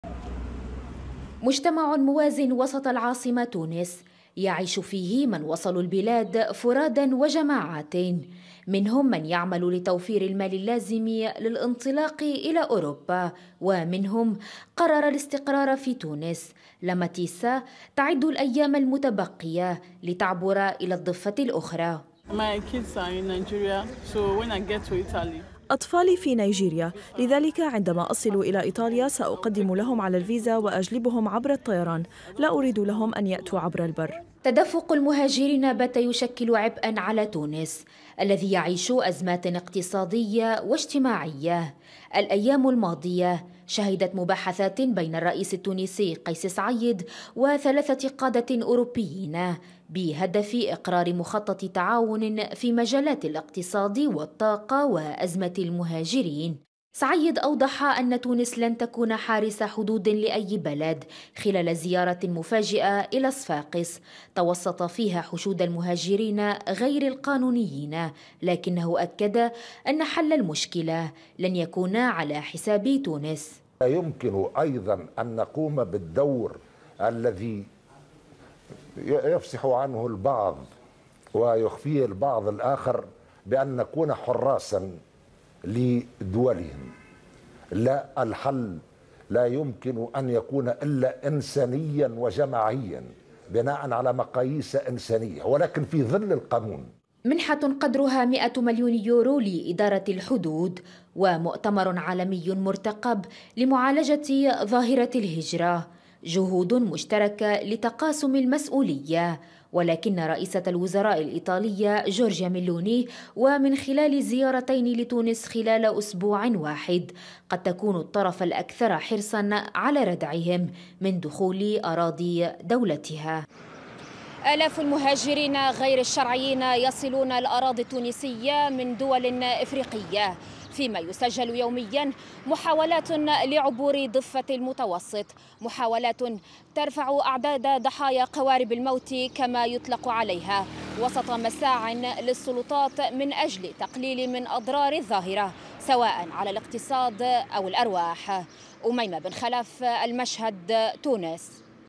استمرار تدفق المهاجرين يشكل عبئا إضافيا على الاقتصاد التونسي
يستمر تدفق المهاجرين غير الشرعيين إلى الدول الأوروبية عبر تونس.. مشكلة كانت محور المحادثات بين الرئيس التونسي والقادة الأوربيين الذين زاروا البلاد مؤخراحرب، حيث تحاول المنظمات المحلية والدولية تجنب وقوع أزمة صحية عبر مبادرات تهدف لمساعدة المرضى

لتحميل تطبيق قناة ومنصة المشهد:
App Store